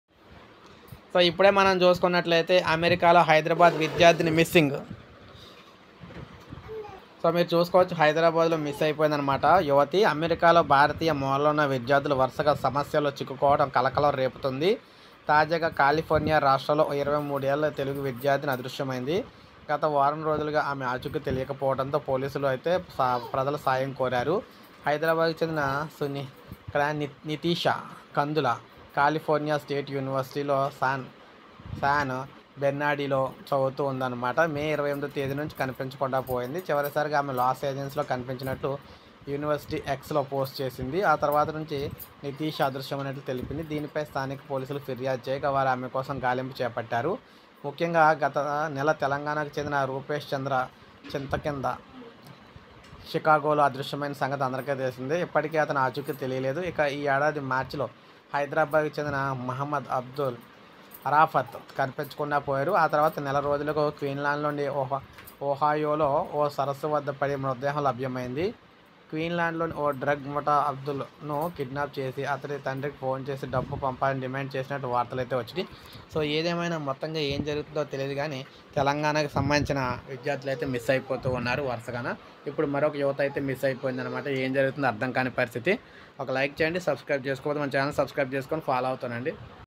మాయమై పోతున్న హైదరాబాద్ విద్యార్థులు big alert for students in Telangana state.
hello everyone today update is TS government Big alert on students.@sarlcreations